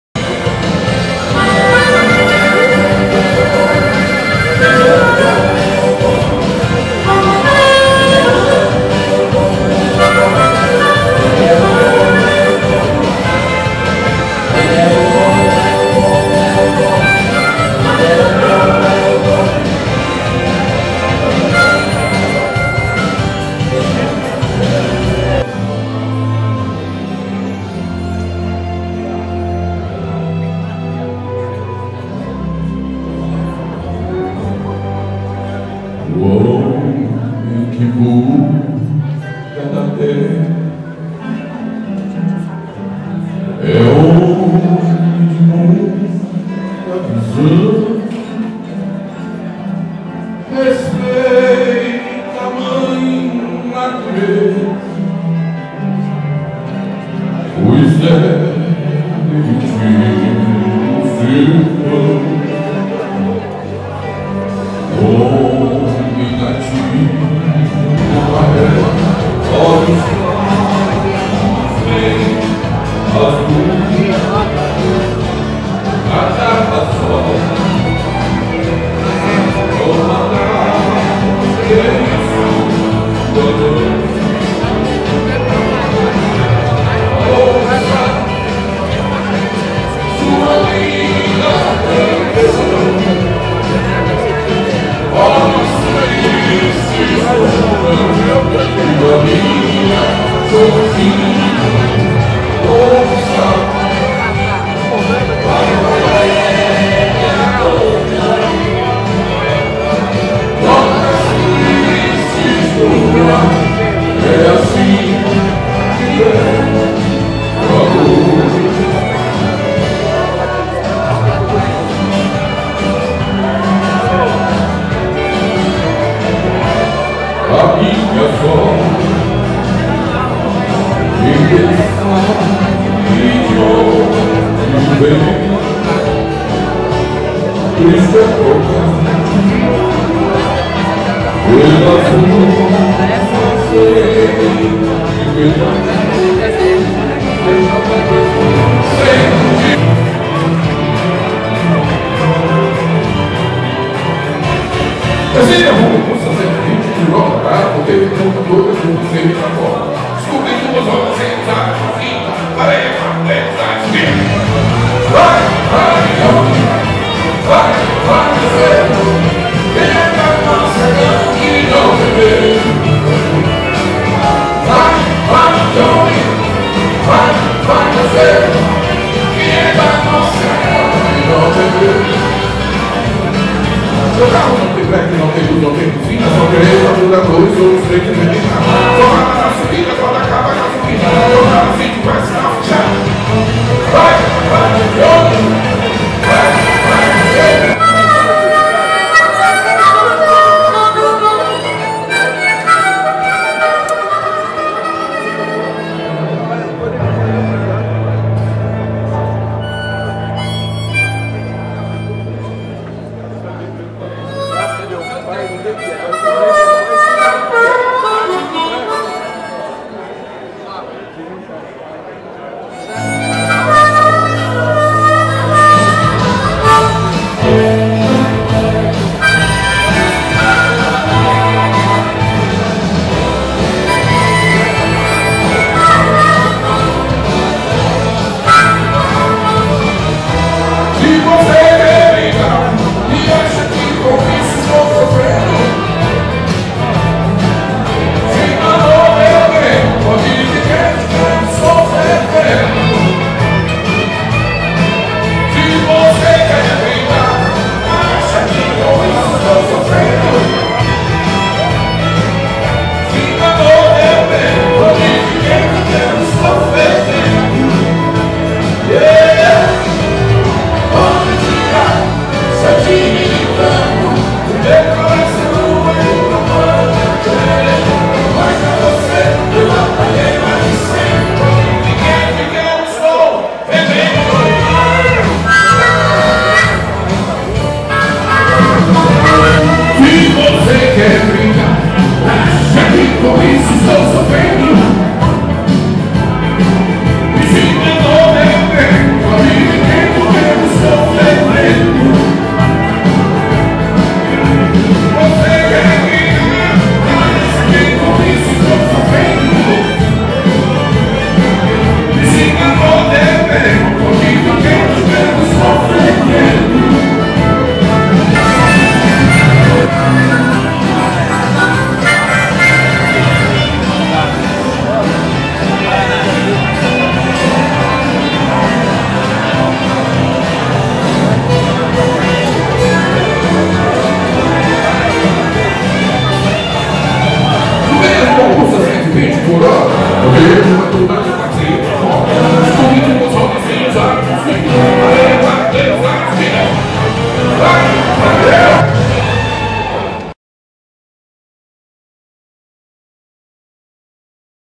Eduardo Araujo
Apresentação no Novotel em São Paulo.
Evento da Faesp.